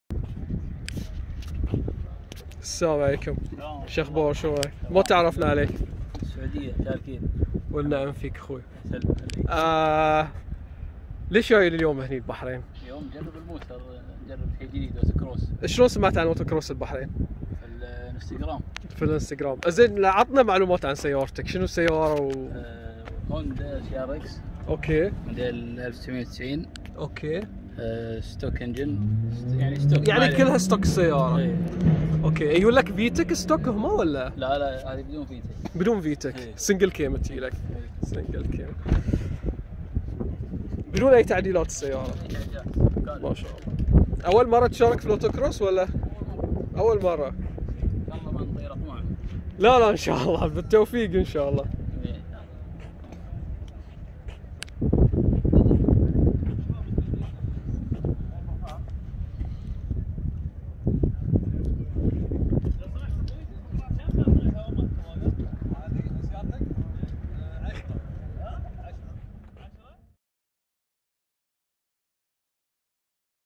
هوندا سي آر اكس 1991 من السعودية - لقاء سريع
احد السيارات اللي لفتت انتباهي
احد المشاركين في اوتوكروس البحرين
ماشاءالله